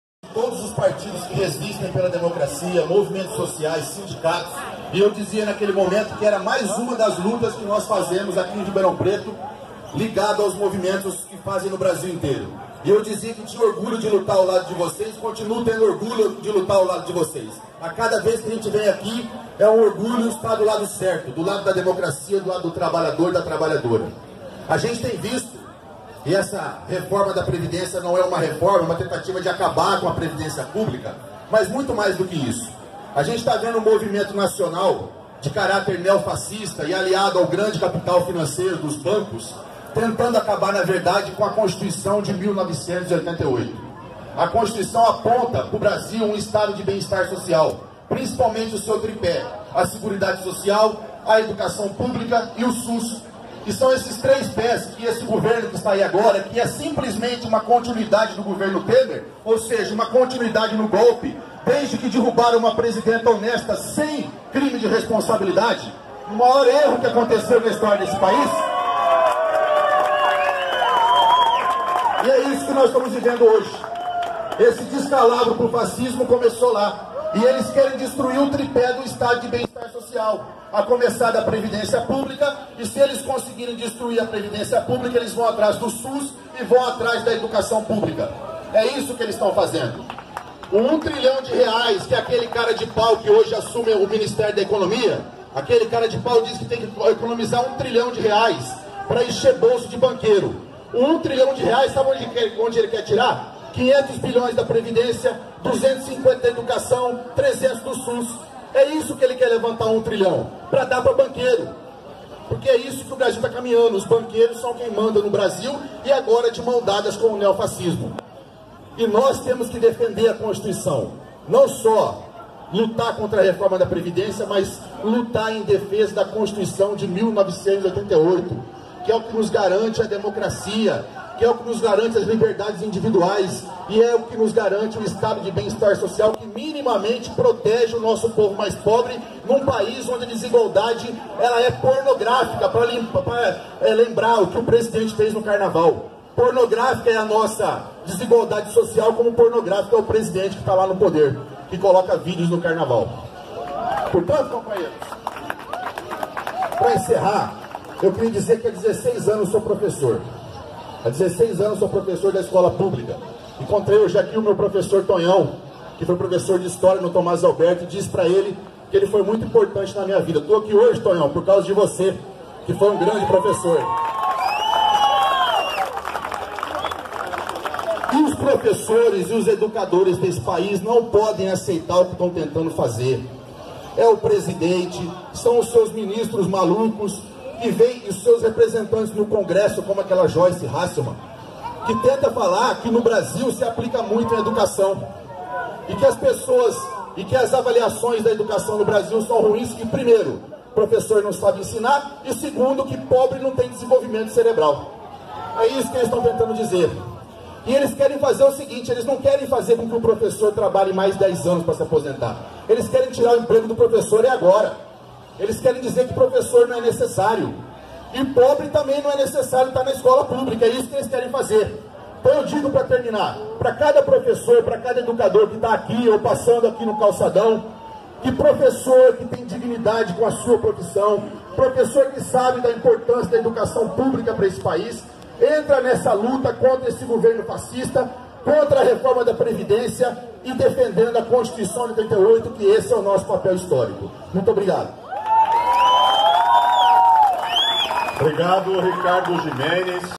Bolsonaro traz o povo progressista de volta à Esplanada - Ricardo Jimenez
Ato contra a reforma da Previdência de Jair Bolsonaro reuniu mais de 300 lideranças na Esplanada Pedro II